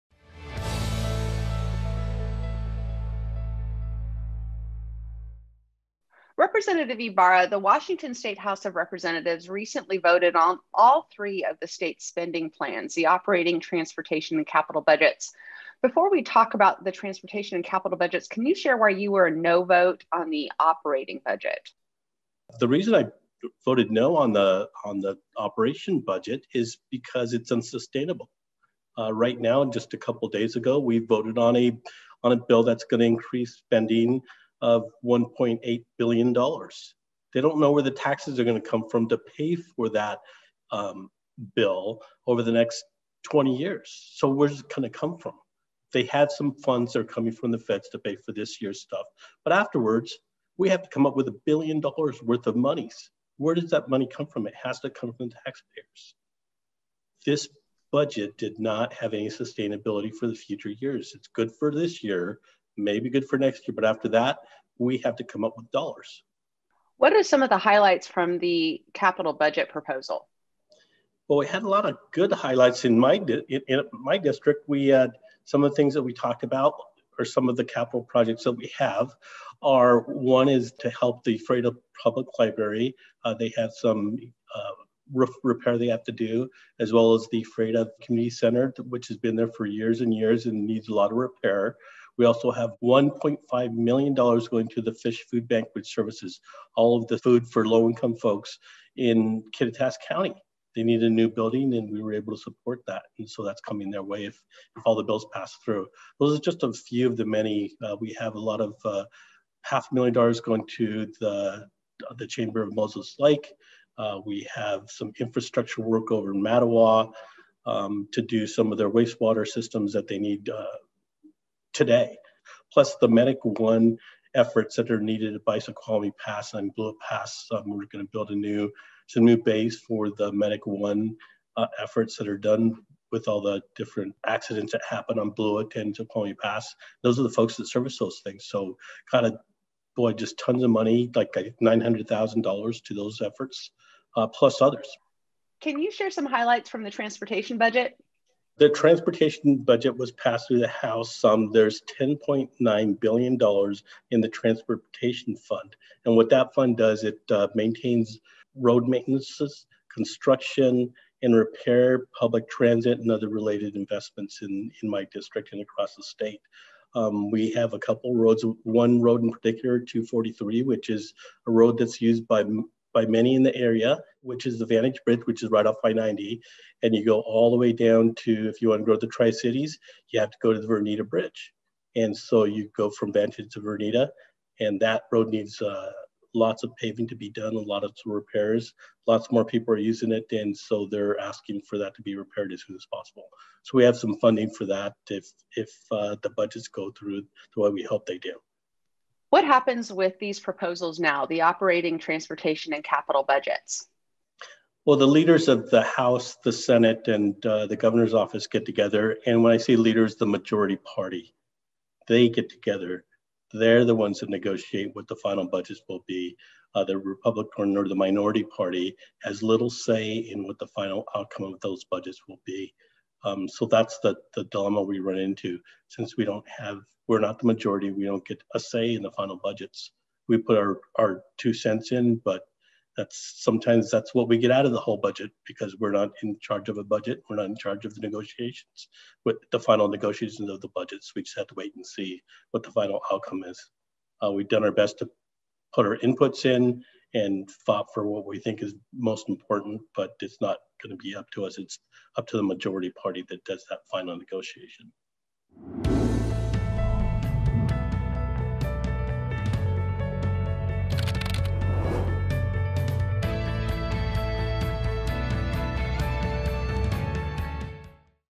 Rep. Alex Ybarra discusses recently approved state spending plans
Rep. Alex Ybarra, R-Quincey, discusses the recent passage of House Democrats' 2021-23 operating, transportation, and capital budgets.